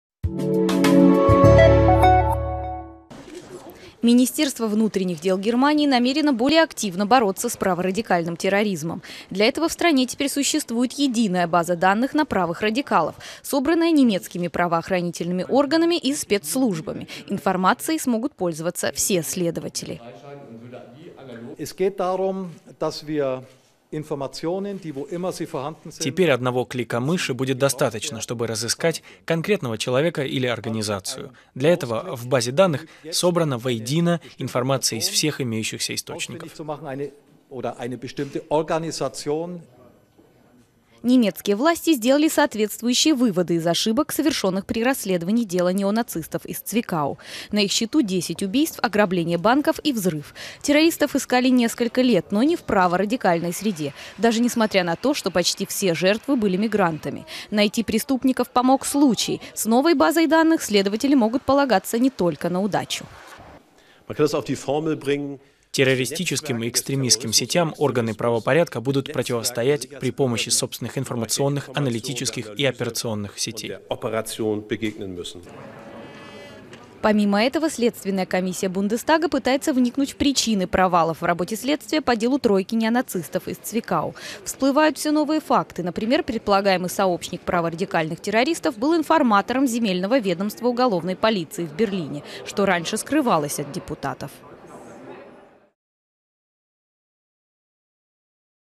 База данных для борьбы с правыми террористами
В новой единой базе данных отныне будет храниться информация о праворадикалах, собранная немецкими правоохранительными органами и спецслужбами. Доступ к ней будет иметь каждый следователь.